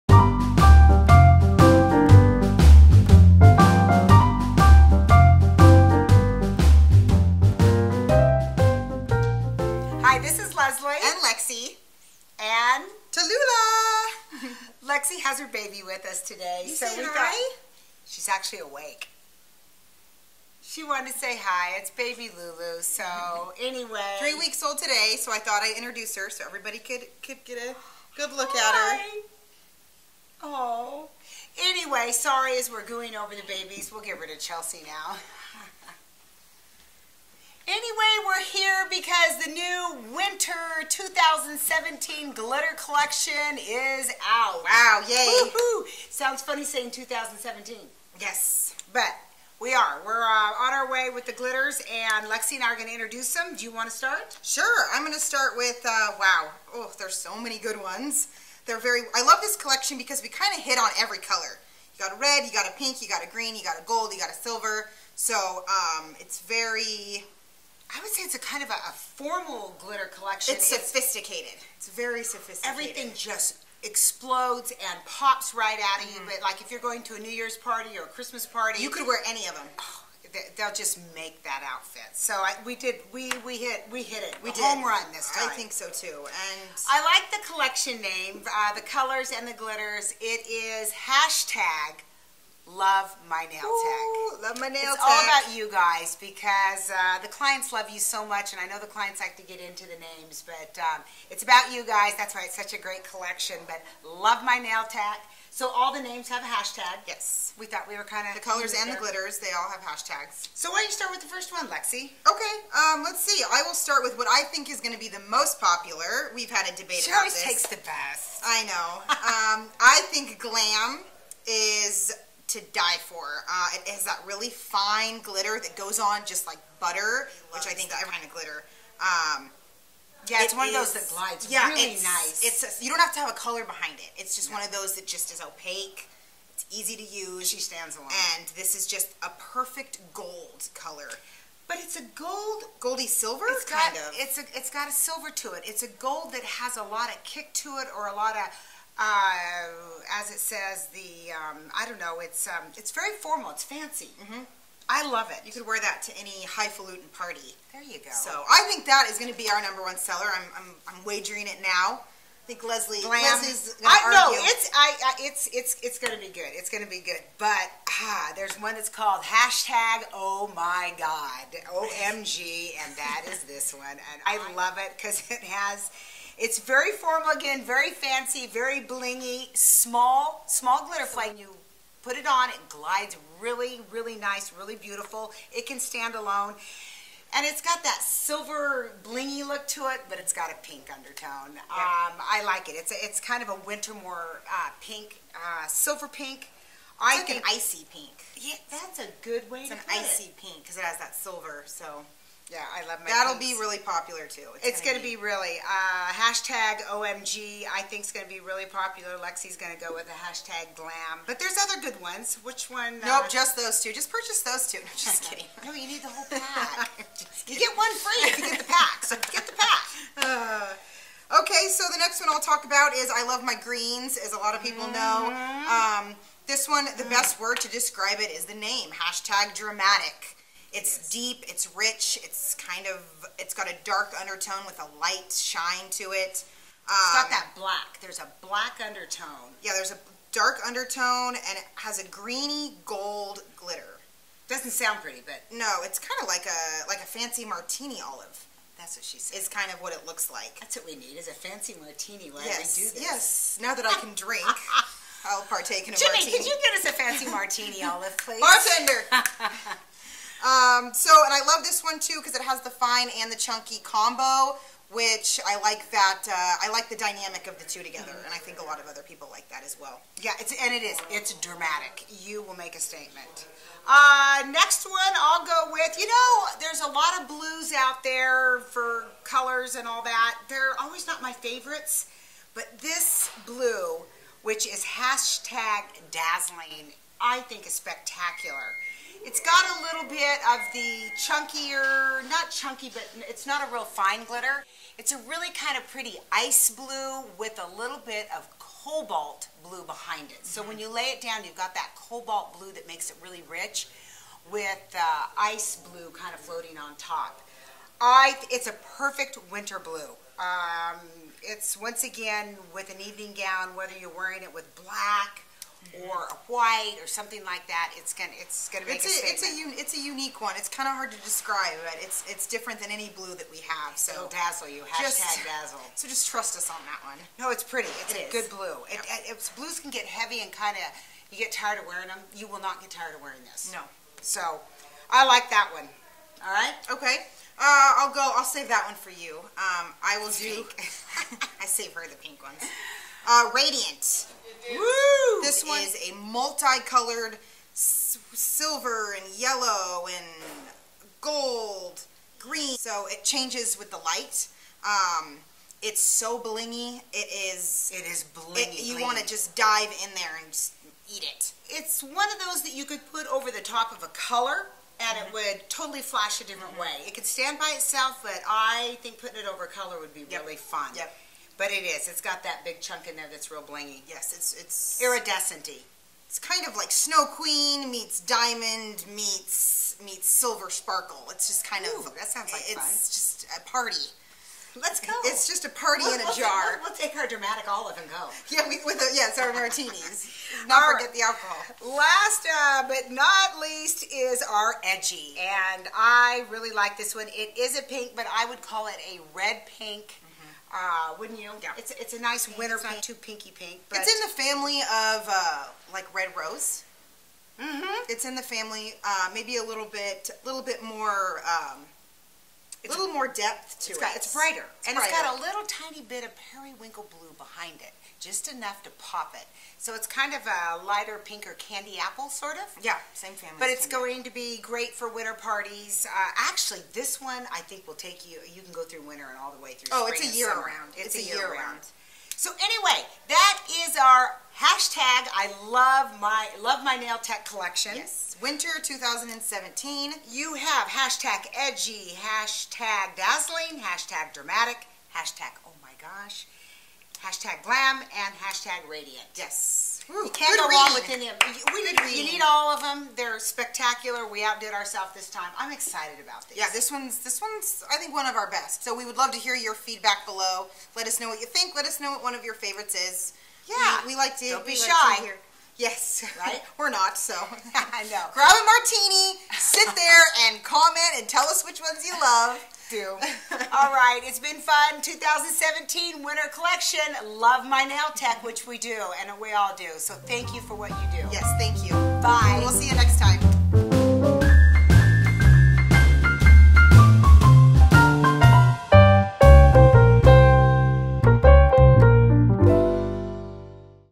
Winter is here! Forecast calls for Glitter.
Lexy and Lezlie introduce the Winter 2017 Glitter Gel collection. This year, the snowflakes are shaped like #, and you'll get to meet all six here.